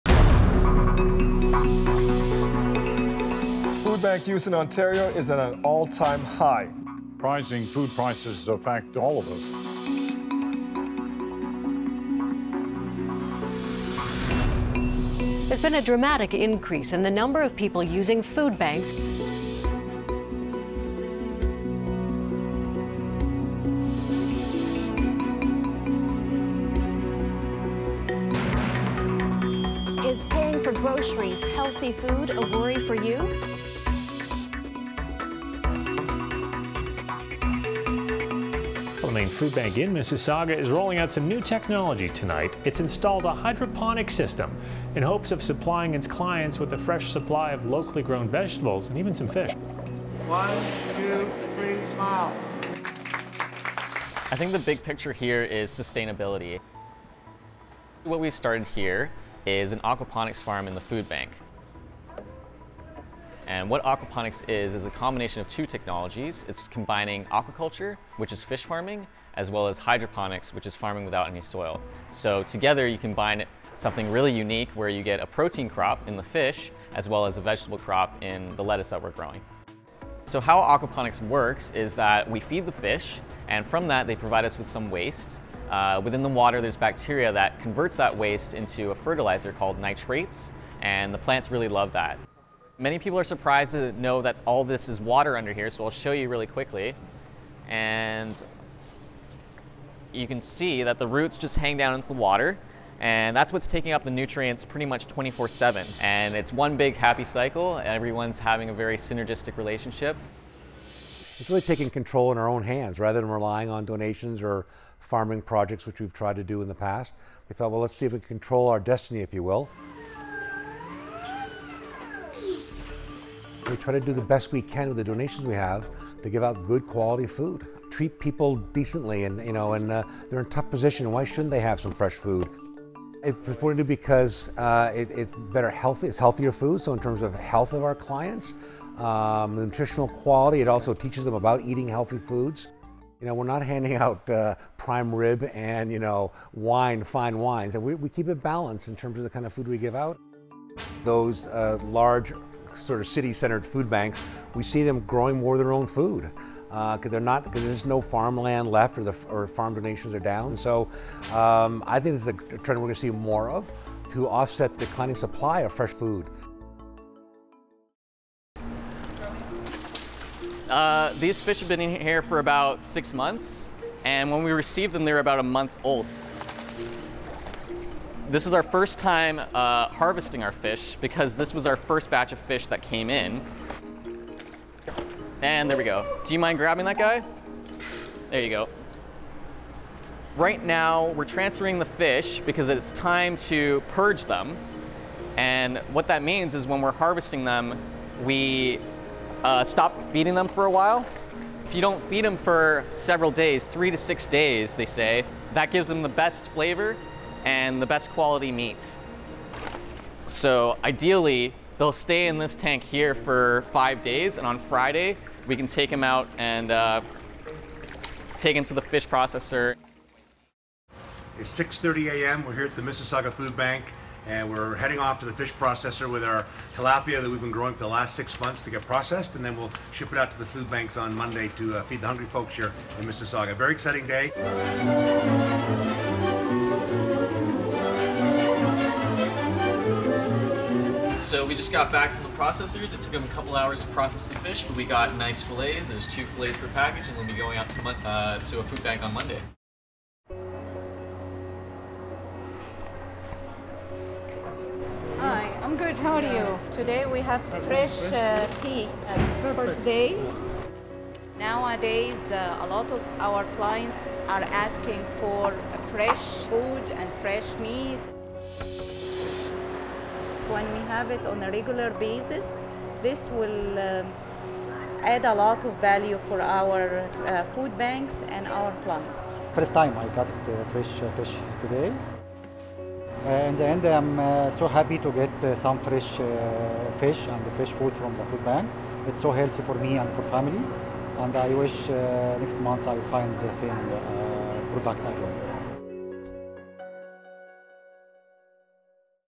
Aquaponic food bank farm
Aquaponics combines raising fish and growing plants in water without soil, and a food bank in the Greater Toronto Area is using it to grow fresh produce locally.